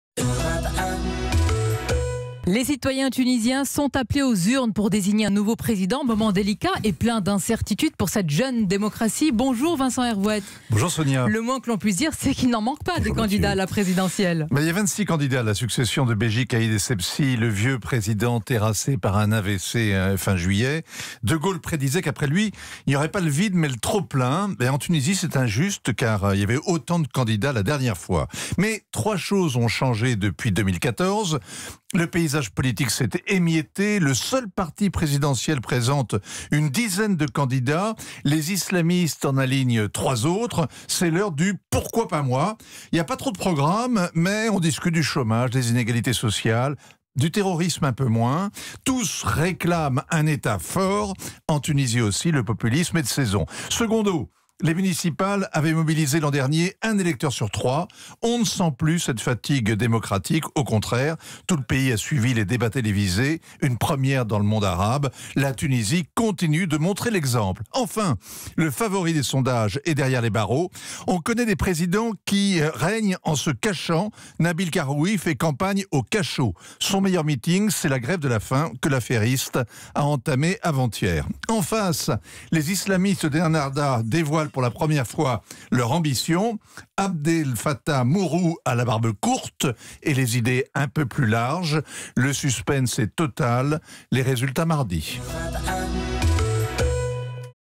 Tunisie : lever de rideau pour les élections de dimanche
Après la mort du président tunisien Beji Caïd Essebsi en juillet, 26 candidats s'affrontent pour lui succéder lors des élections ce dimanche.